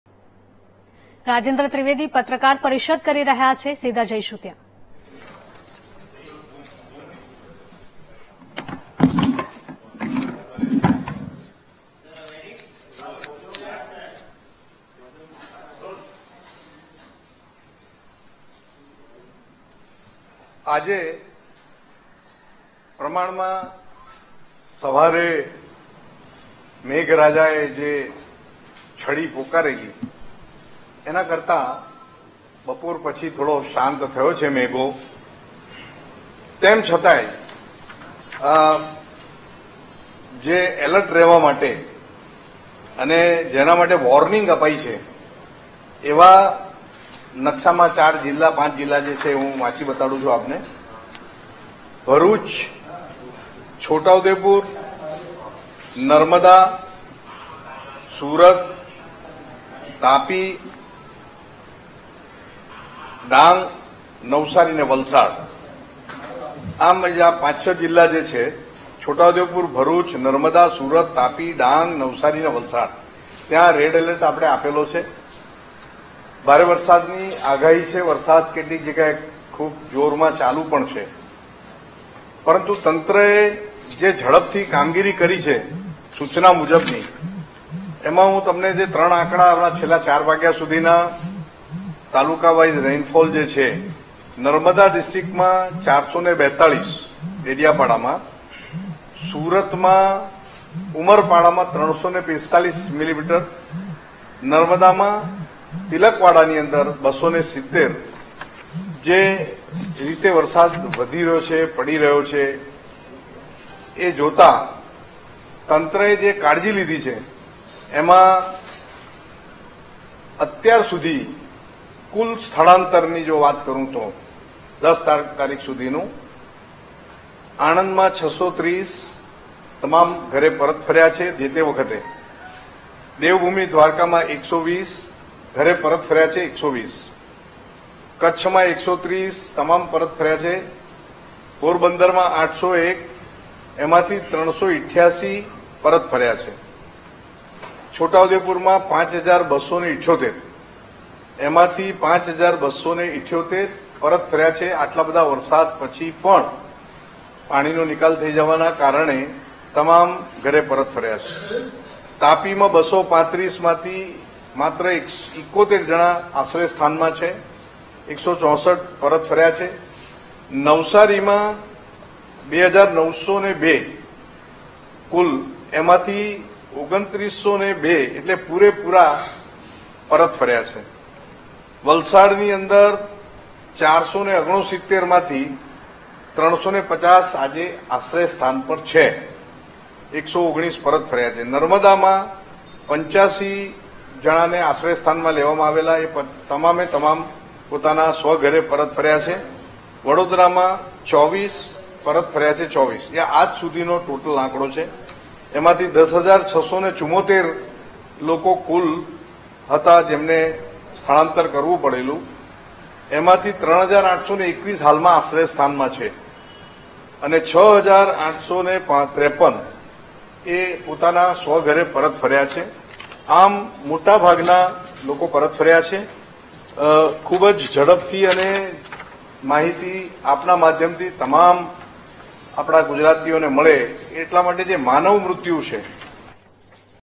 Gujarat માં 24 કલાકમાં 508 માંથી 468 નાગરિકો ઘરે પરત ફર્યા: Rajendra Trivedi
ગુજરાતની તારાજીની સ્થિતિનો તાગ: કુલ 10 હજારને રેસ્ક્યૂ કરાયા, 63ના મોત, આ 7 જિલ્લામાં રેડ એલર્ટ જાહેર